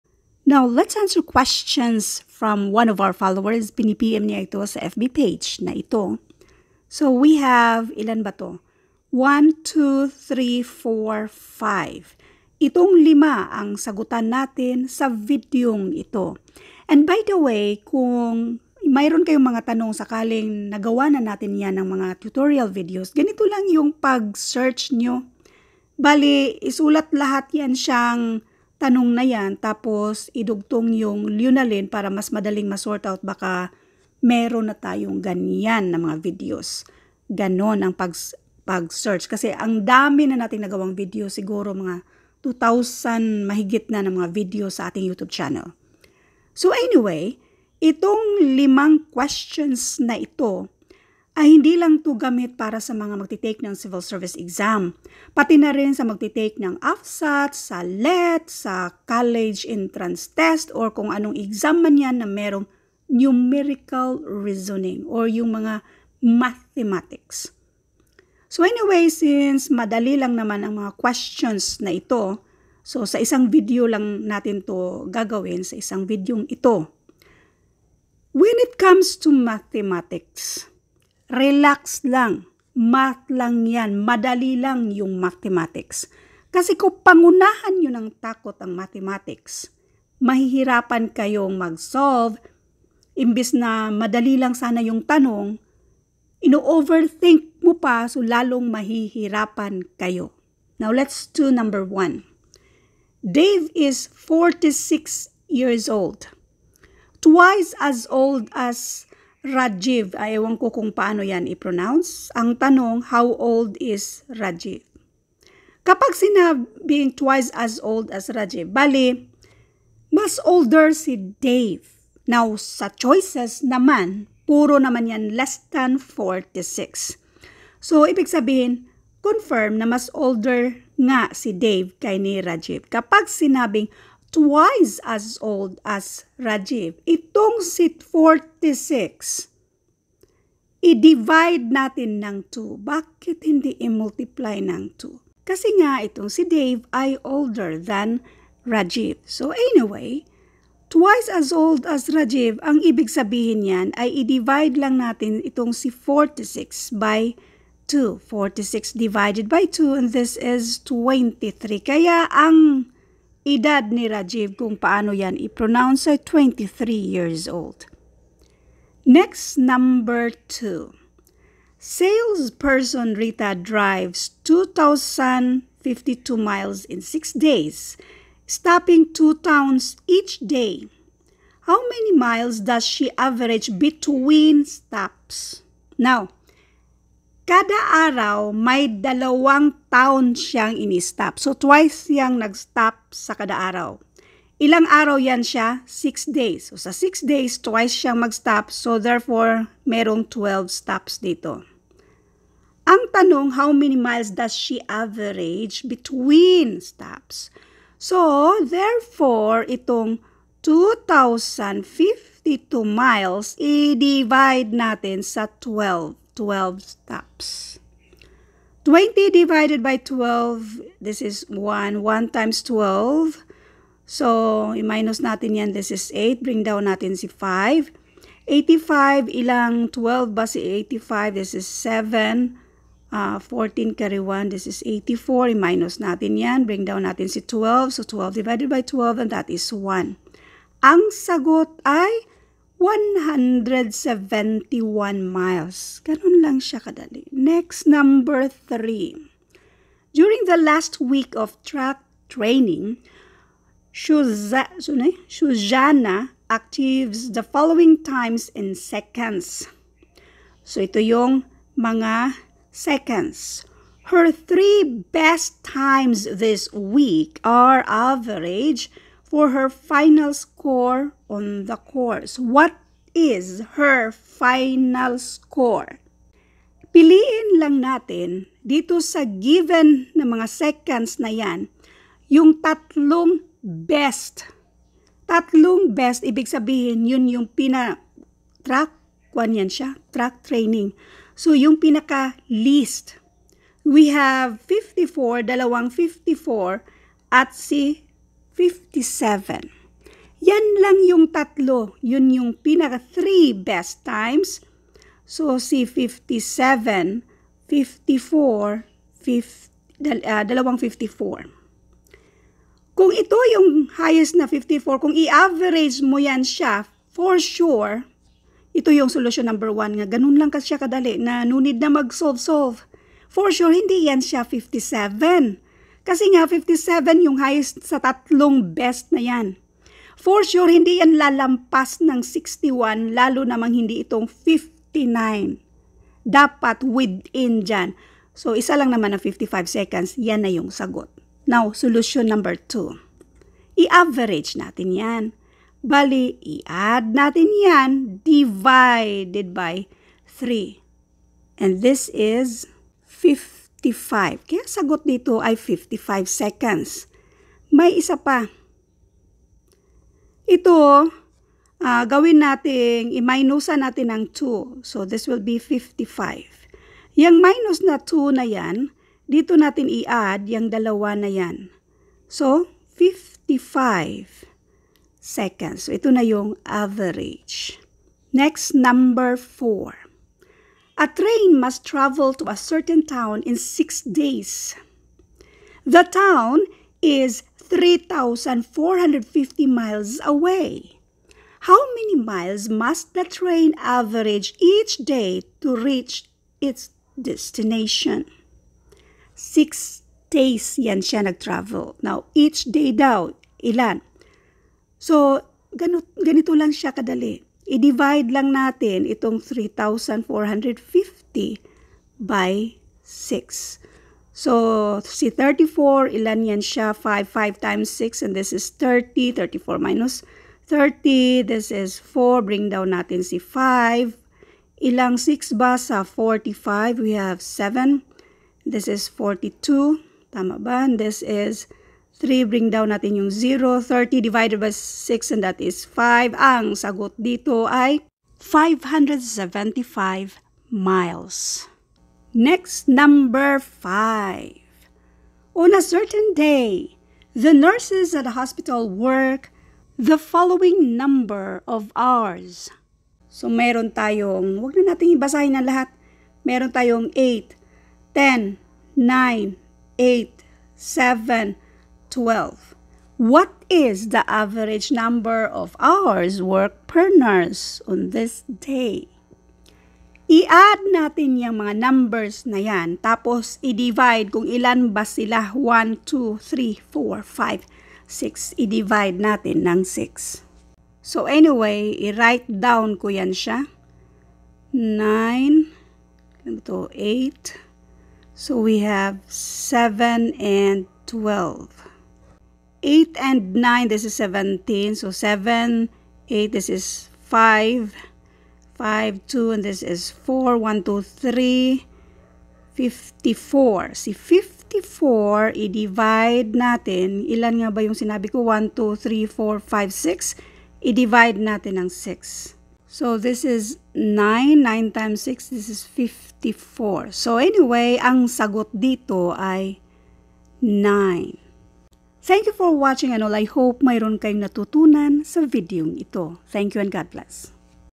5-item Numerical Reasoning
1.) Dave is 46 years old, twice as old as Rajeeve. How old is Rajeeve?
a. 30 yrs old
b. 28 yrs old
c. 23 yrs old
d. 18yrs old

2.) Salesperson Rita drives 2,052 miles in 6 days, stopping at 2 towns each day. How many miles does she average between stops?
a. 171 miles
b. 342 miles
c. 684 miles
d. 1,026 miles

3.) During the last week of track training. Shoshanna achieves the following times in seconds: 66, 57, 54, 54, 64, 59, and 59. Her three best times this week are averages for her final score on the course. What is her final score?
a. 57 seconds
b. 55 seconds
c. 59 seconds
d. 61 seconds

4.) A train must travel to a certain town in six days. The town is 3,450 miles away. How many miles must the train average each day to reach its destination?
a. 500 miles
b. 525 miles
c. 540 miles
d. 575 miles

5.) On a certain day, the nurses at a hospital worked the following number of hours: Nurse Howard worked 8 hours, Nurse Pease worked 10 hours, Nurse Campbell worked 9 hours, Nurse Grace worked 8 hours, Nurse McCarthy worked 7 hours and, Nurse Murphy worked 12 hours. What is the average number of hours worked per nurse on this day?
a. 7
b. 8
c. 9
d. 10